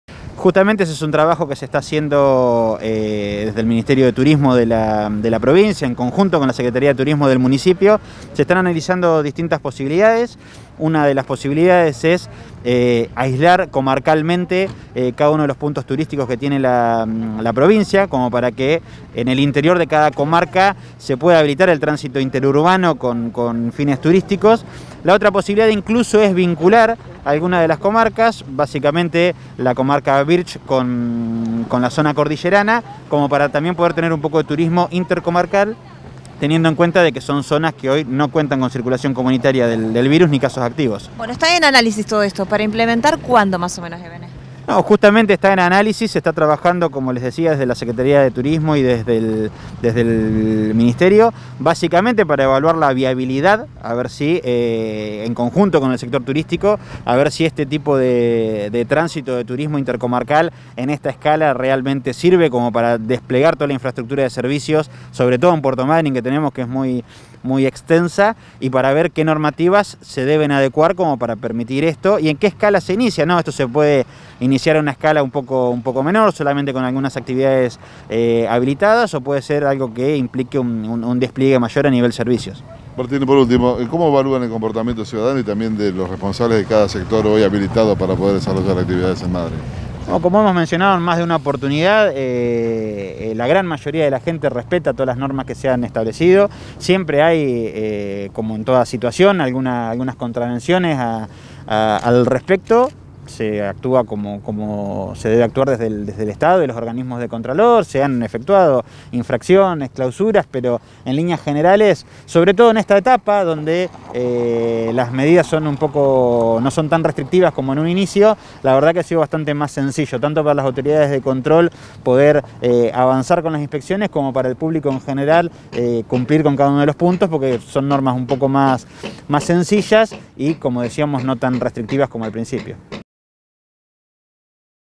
Analizan la habilitación de actividades turísticas con protocolos.
Nota: Martín Ebene, secretario de Gobierno de Puerto Madryn.